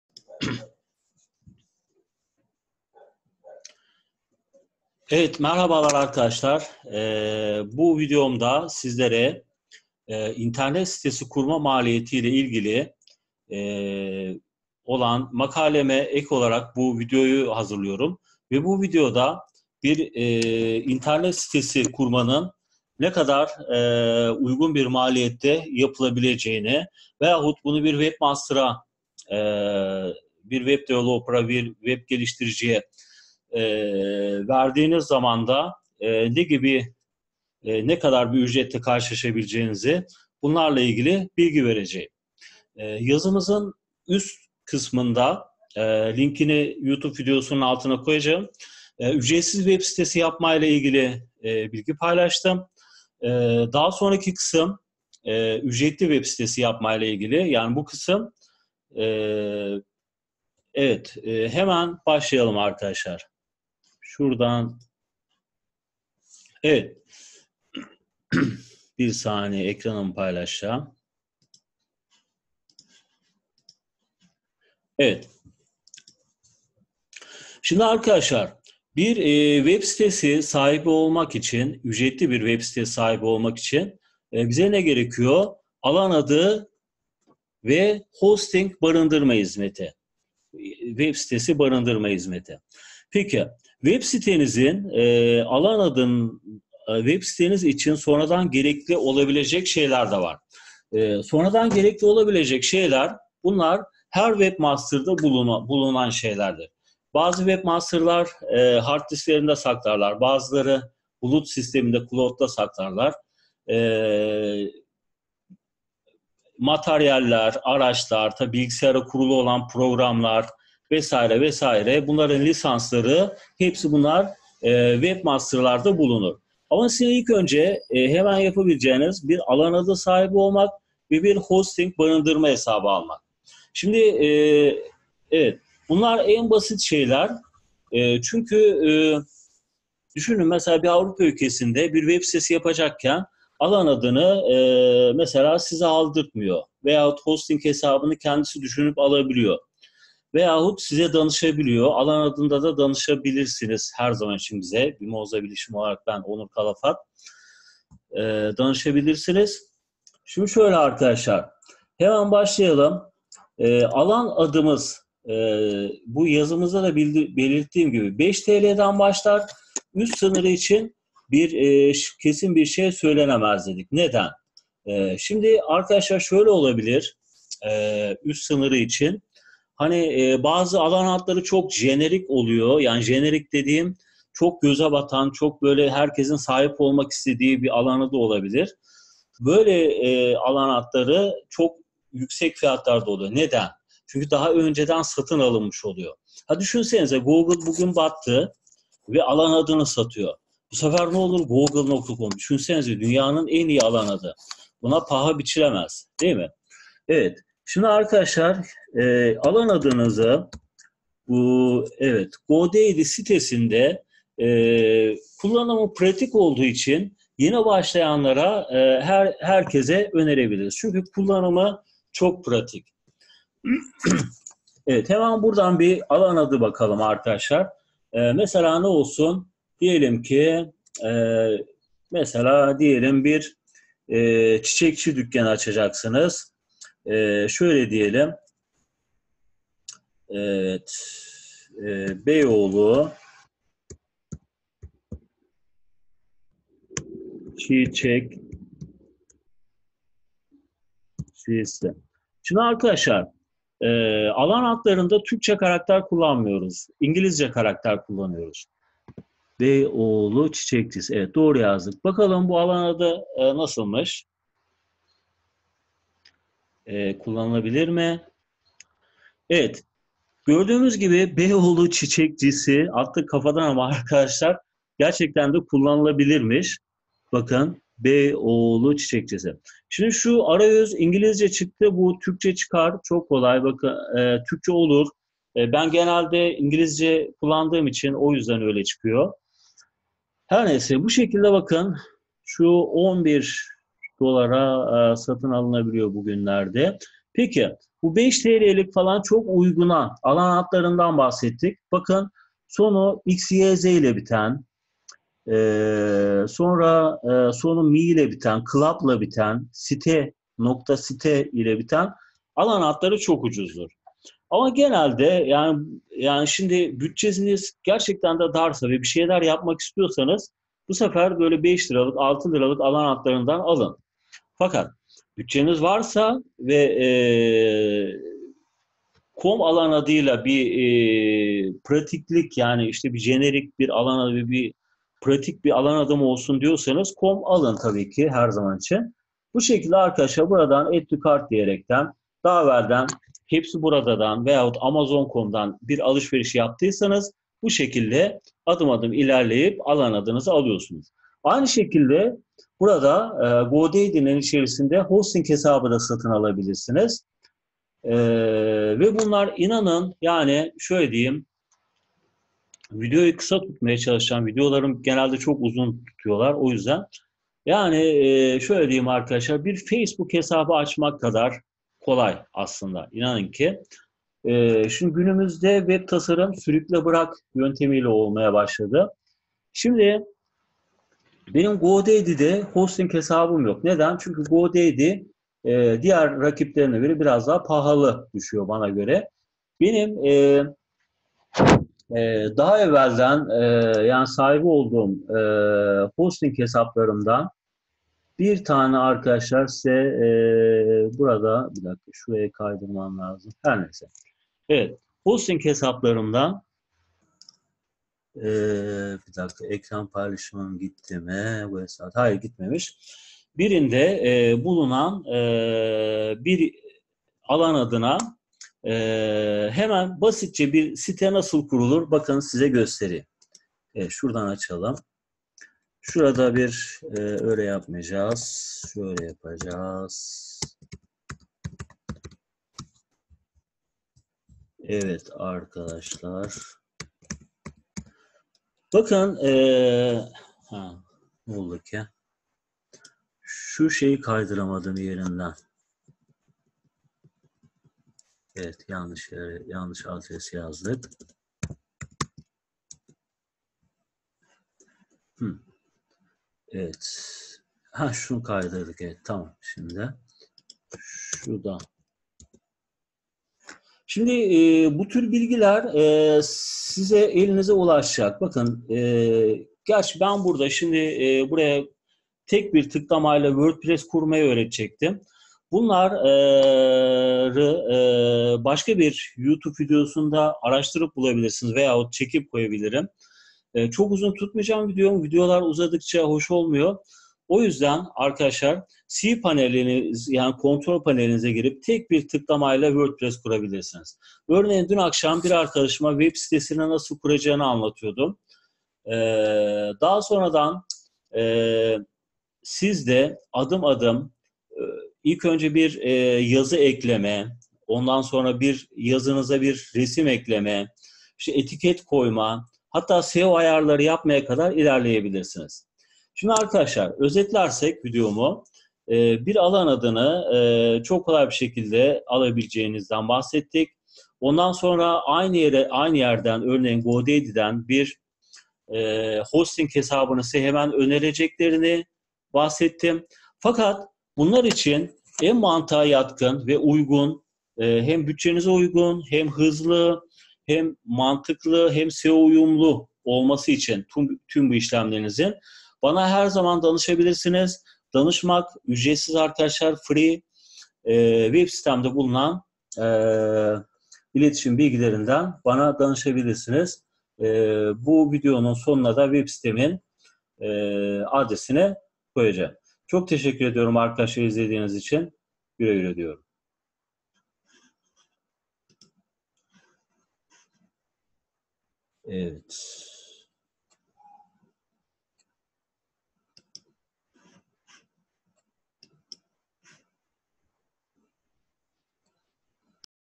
Alan Adı ve Hosting Satın Alma
Alan adı ve hosting (barındırma hesabı) nasıl satın alınır